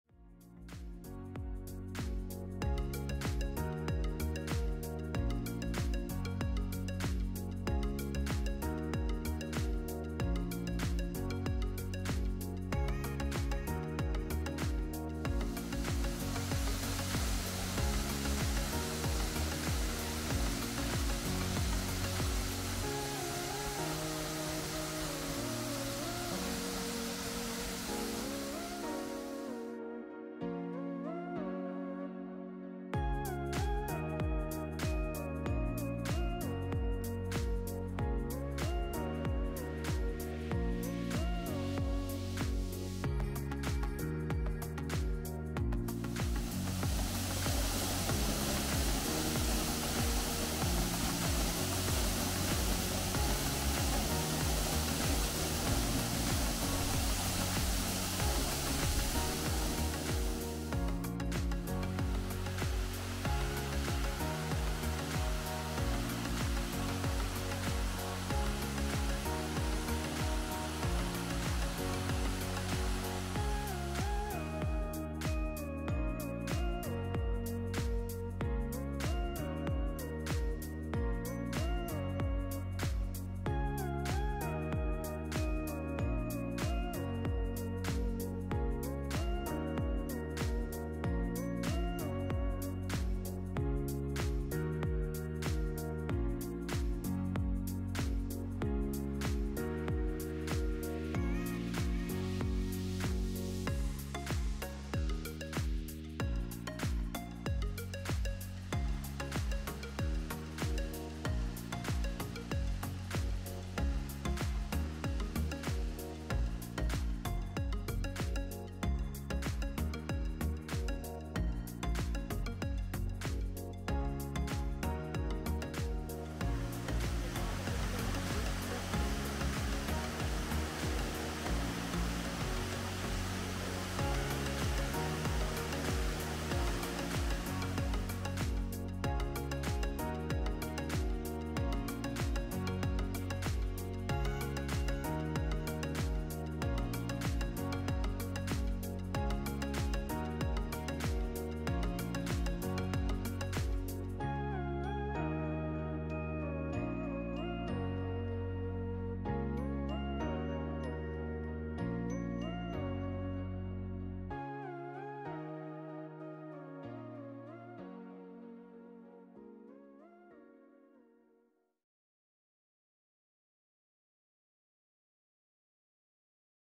Cascade du Queureuilh, Rossignolet & La Grande Cascade du Mont-Dore, Le Mont-Dore, France [4K]
A short impression of a visit to 3 waterfalls that are close together near Le Mont-Dore in Auvergne-Rhône-Alpes, France:

[Cascade du Queureuilh]
Picturesque waterfall on the Ronde des Cascades hiking path with a 20-m. plunge over basalt columns.

[Cascade du Rossignolet]
Scenic waterfall, reached by a shaded path, with a veil-like cascade over a volcanic ash deposit.

[Grande cascade du Mont-Dore]
A 30m waterfall just outside the village of Mont Dore. Mont Dore is located south west of Clermont Ferrand in the region Auvergne/Puy de Dome, France. La Grande cascade du Mont Dore has its source at the plateau Balayé at an altitude of 1.450 meters.

[ PHOTOS ]
Olympus OM-D E-M5 Mark II
Olympus M.Zuiko Digital ED 12-45mm F4 PRO

[ COPYRIGHT ]
J.Oerlemans (DaNaca)